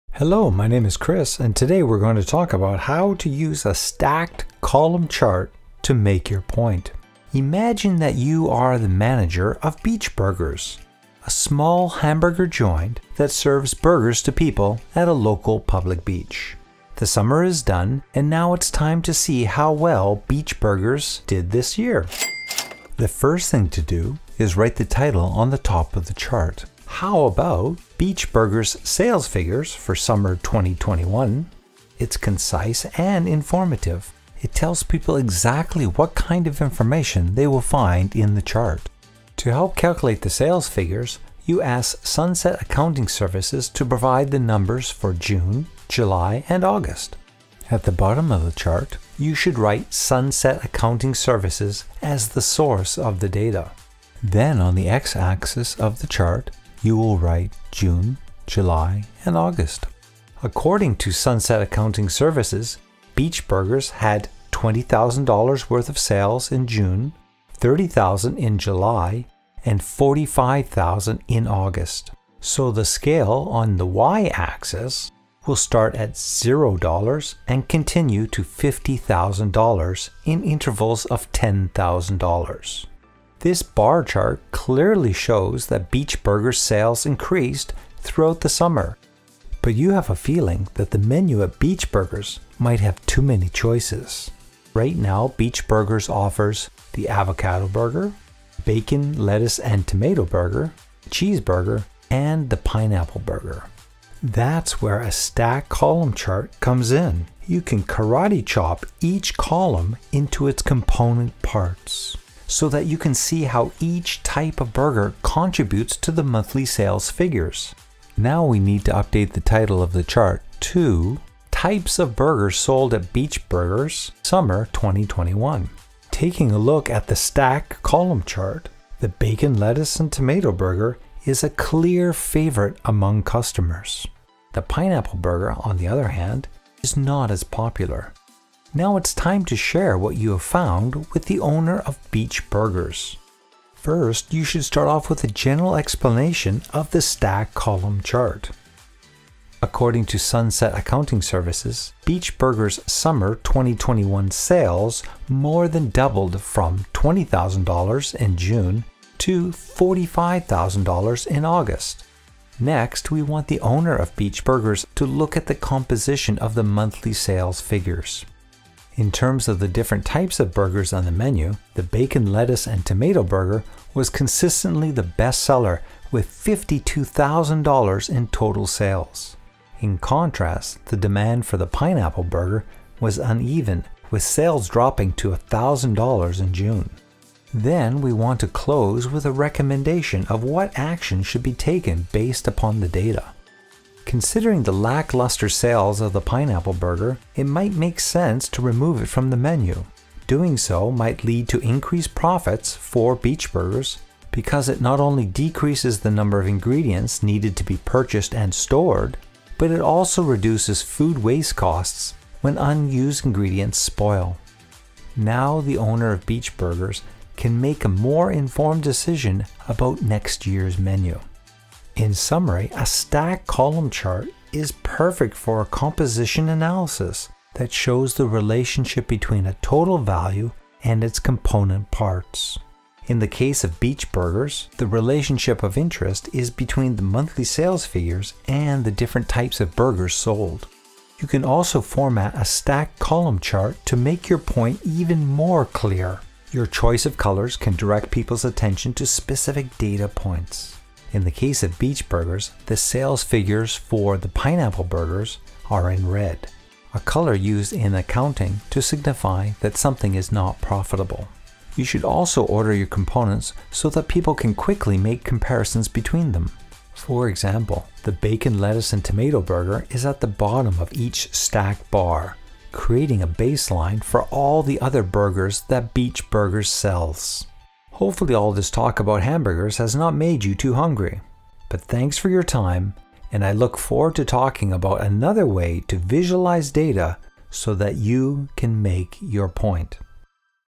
Use a Stacked Column Chart to Explain Your Data
Today will we talk about how to use a stacked column chart for a compositional analysis. Don't worry. It can be done with a three-part description and analysis.  Here is an example for a fictitious independent burger shop.

"According to Sunset Accounting Services, Beach Burgers’ Summer 2021 Sales more than doubled from $20,000 in June to $45,000 in August. In terms of the different types of burgers on the menu, the Bacon, Lettuce and Tomato Burger was consistently the best seller ($52,000 in total sales). In contrast, demand for the Pineapple Burger was uneven with sales dropping to $1,000 in June. Considering the lackluster sales of the Pineapple Burger, it might make sense to remove it from the menu. Doing so, might lead to an increased profits for Beach Burgers because it not only decreases the number of ingredients needed to be purchased and stored, but it also reduces food waste costs when unused ingredients spoil."